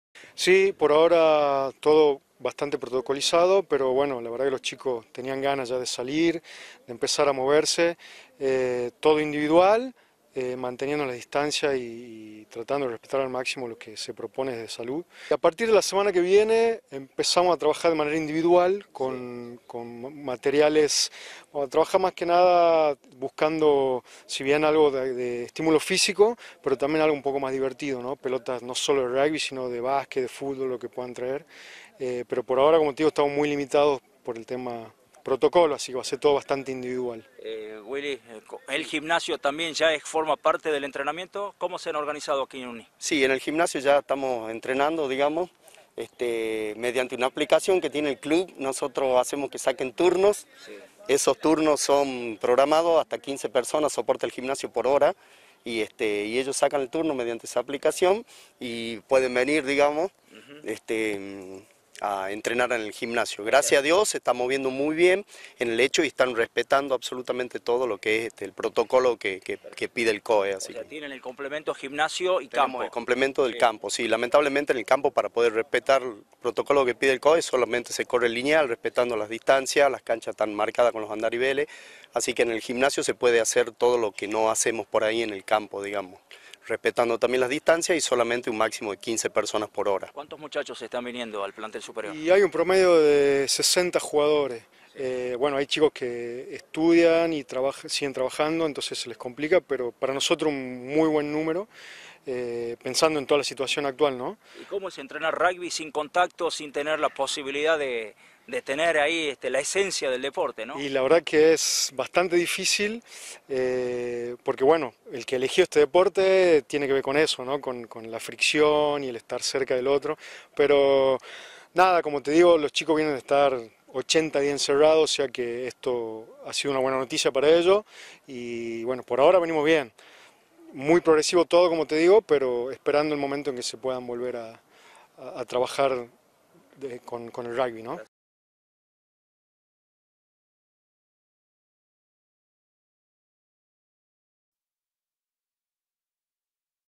UNI de Tucumán volvió a los entrenamientos
Universitario de Tucumán, actual Campeón del Torneo Regional del NOA, volvió a los entrenamientos (29/06/20).